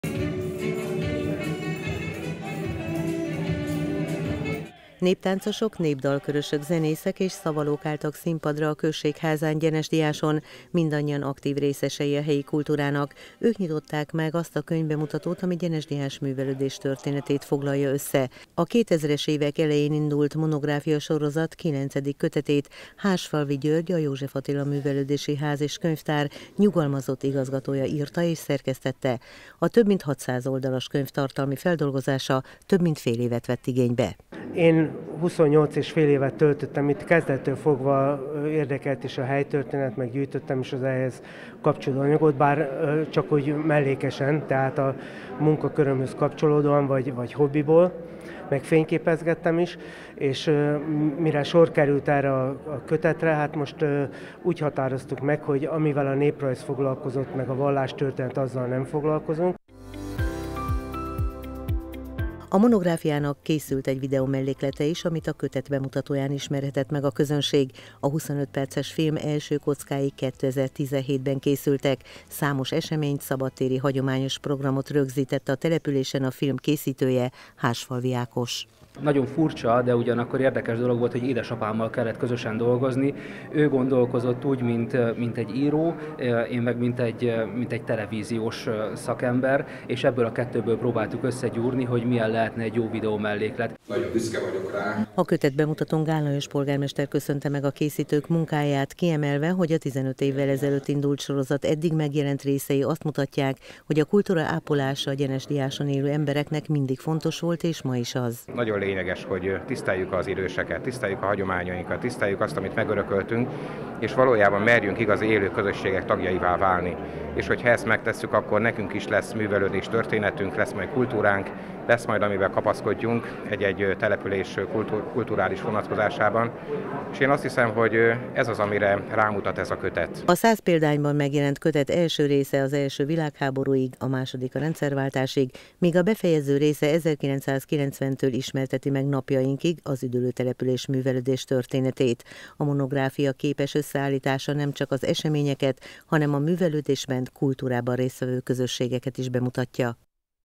Gyenesdiás művelődéstörténete
Gyenesdiás művelődéstörténetéről szól a település monográfia sorozatának kilencedik része. A Balaton nyugati szélén fekvő üdülő település életében mindig nagy szerepet kapott a kultúra, a helyi művelődési közösségek szerveződése és munkája. Több mint 100 évet ölel fel a kötet. A könyv és a hozzá kapcsolódó kisfilm bemutatójára tegnap került sor Gyenesdiáson.